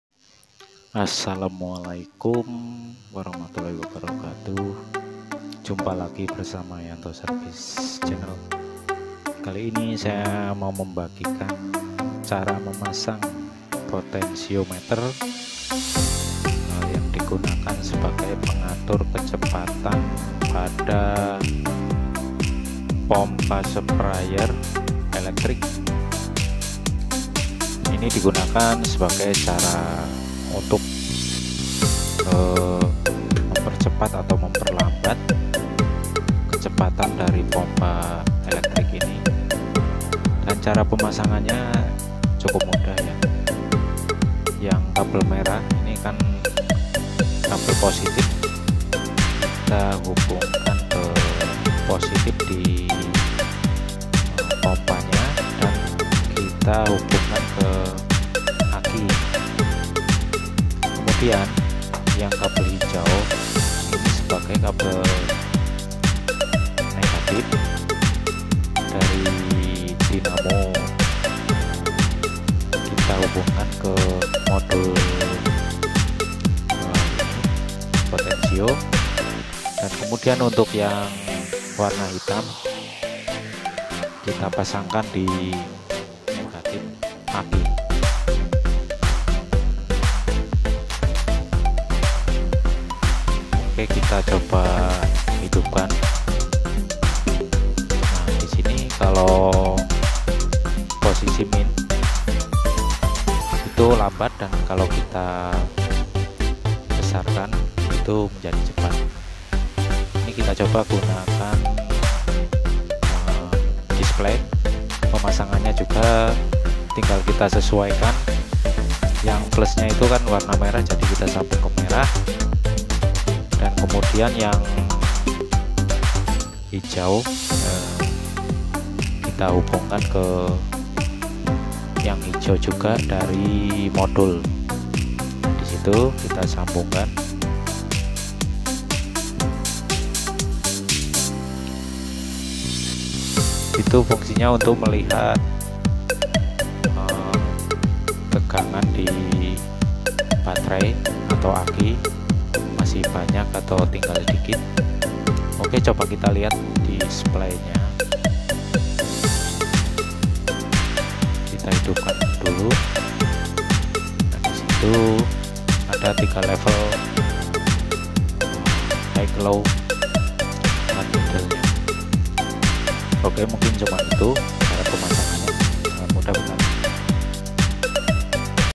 Cara memasang potensiometer pada sprayer electric
Sering kali pada pompa sprayer,terjadi kerusakan pada dimmer potensionya sehingga pompa sprayernya nya tidak hidup ataupun tidak bekerja ,dalam postingan ini saya bagikan cara menyambungnya atau merangkainya saja semoga bermanfaat.